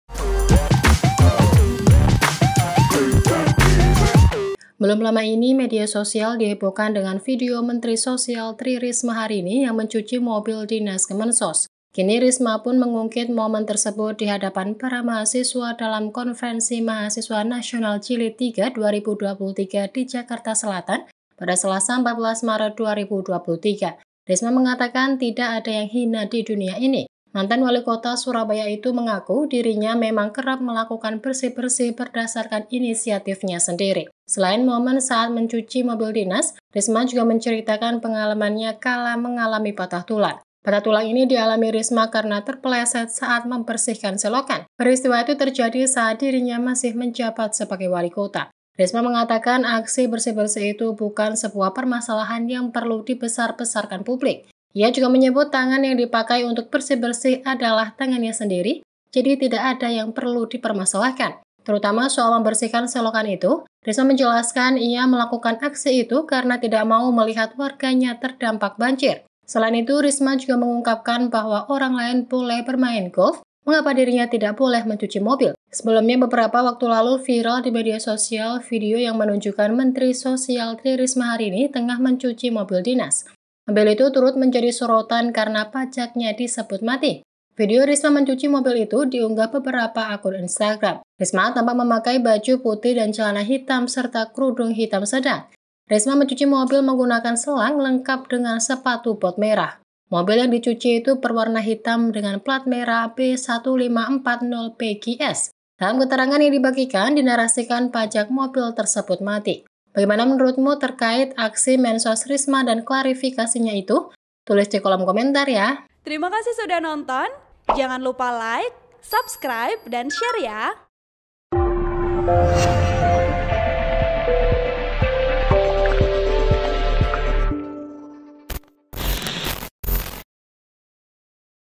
Mensos Risma Singgung soal Cuci Mobil Dinas Viral dan Momen Patah Tulang saat Bersihkan Selokan
Menurut Risma tidak ada yang hina di dunia ini.

Adapun hal itu disampaikannya pada Konferensi Mahasiswa Nasional Jilid III 2023 dengan tema “Optimalisasi SDM Unggul Demi Tercapainya Sustainable Development Goals (SDGs) 2030 di Indonesia, Jakarta Selatan, Selasa (14/3/2023).

"Sama siram-siram ya bersih-bersih mobil. Bagi saya apa yang hina di dunia ini. Saya jadi walikota tangan saya patah, saya bersihkan selokan lalu ke pleset jadi tangan saya patah," kata Risama kepada mahasiswa.

Risma melanjutkan apa yang salah dengan tangannya sendiri melakukan kebaikan.

"Apa yang masalah tangan saya sendiri. Saya tidak ingin warga saya nggak kena banjir, apa yang salah," jelasnya.

Adapun saat dikonfirmasi setelah acara berakhir, Risma mengungkapkan bahwa orang lain boleh bermain golf mengapa dirinya tidak boleh mencuci mobil.

"Orang boleh main golf kenapa saya nggak boleh cuci mobil," tegasnya.

Mobil itu turut menjadi sorotan karena pajaknya disebut mati.

Video Risma mencuci mobil itu diunggah beberapa akun Instagram.

Risma tampak memakai baju putih dan celana hitam serta kerudung hitam sedang.

Risma mencuci mobil menggunakan selang lengkap dengan sepatu bot merah.

Mobil yang dicuci itu berwarna hitam dengan pelat merah B-1540-PQS.

Dalam keterangan yang dibagikan, dinarasikan pajak mobil tersebut mati.

Artikel ini telah tayang di Tribunnews.com dengan judul Singgung Foto Cuci Mobil Dinas, Risma: Orang Boleh Main Golf Kenapa Saya Nggak Boleh Cuci Mobil,
Penulis: Rahmat Fajar Nugraha
Editor: Johnson Simanjuntak
VO: Jayanti Tri Utami
VP: Fegi Sahita